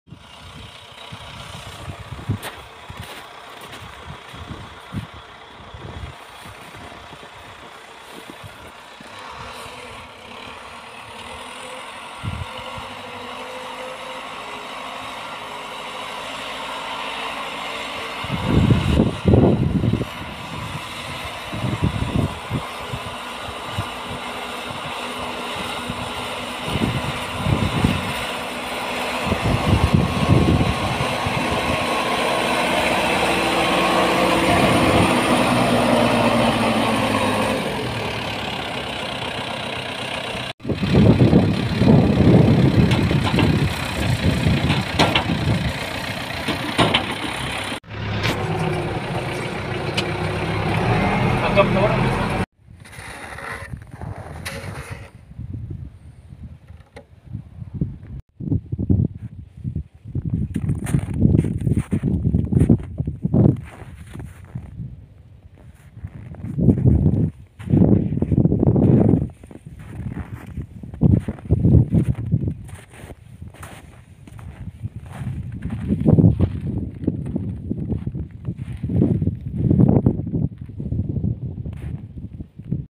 Қар тау. Снежная горка. Snow slide 🏔🚜❄farm
Бір АЛЛАНЫҢ қалауымен ауылда балаға арнап горка жасадық.